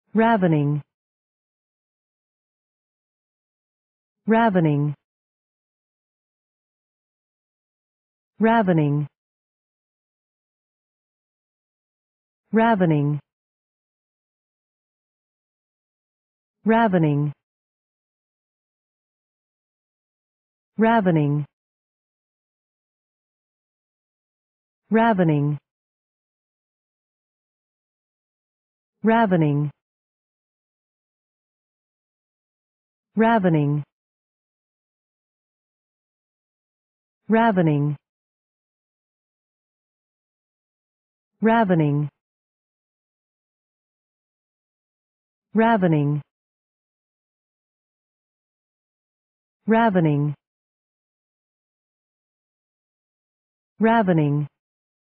How to pronounce Ravening | English pronunciation
Learn how to pronounce Ravening in English
RAVENING
Pronunciation of Ravening: /'rævənɪŋ/
Definition of Ravening: (1) (p. pr. & vb. n.) of Raven (2) (n.) Eagerness for plunder; rapacity; extortion. (3) (a.) Greedily devouring; rapacious; as, ravening wolves.